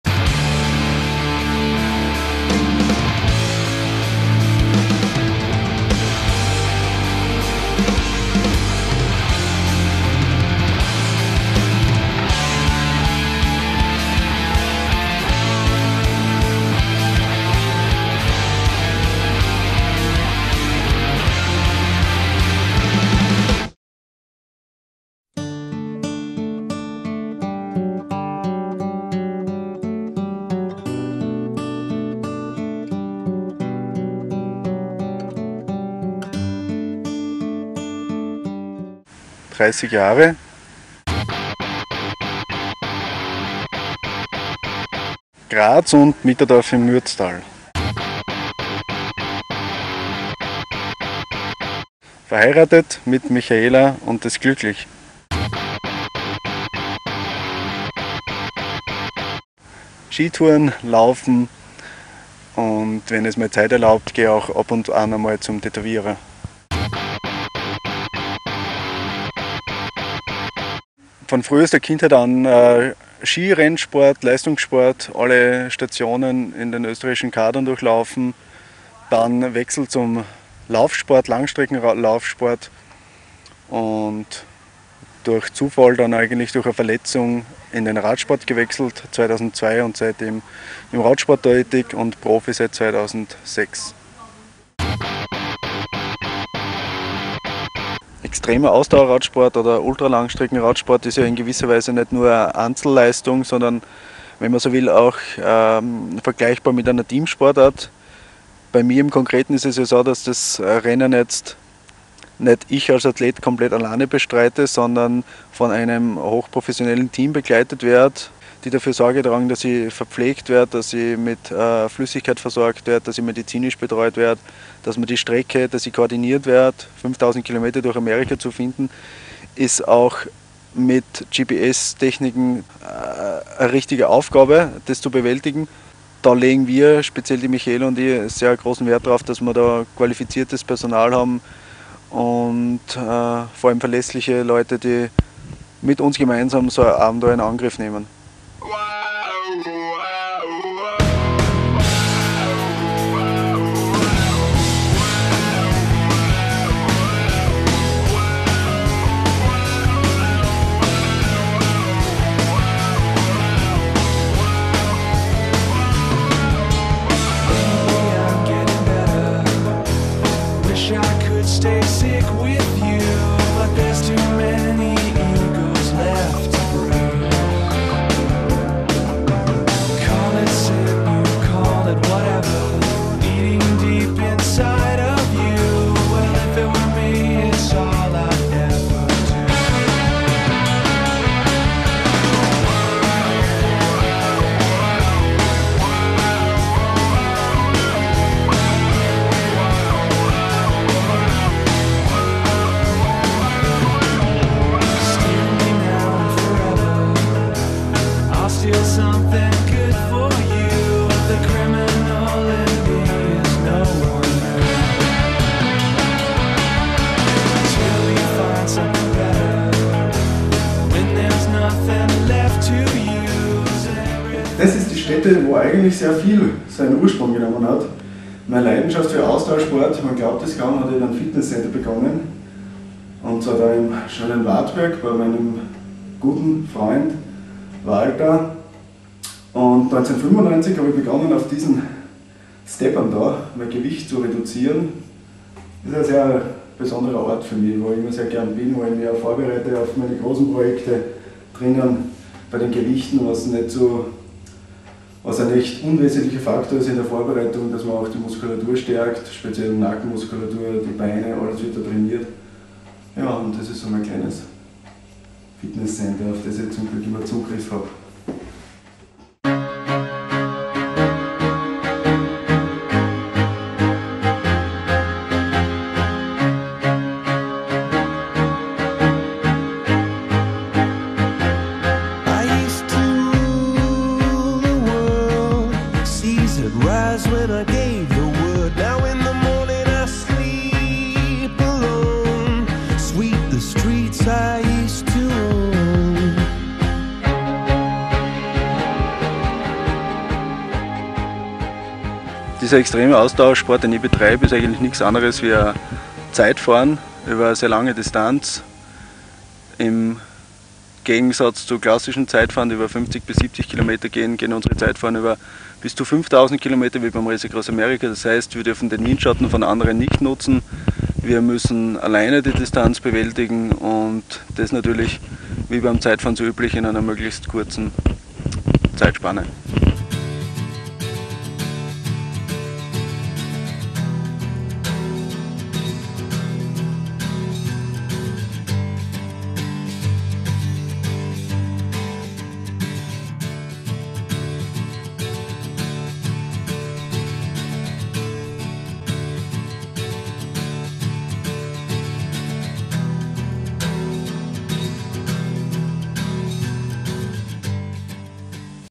Franz Preihs (RAAM Finisher 08) Videoportrait Part 1
Teil 1 des Videoportraits vom Extremradsportler Franz Preihs.
   -   Part 1 from the video portrait of ultra endurance athlet Franz Preihs.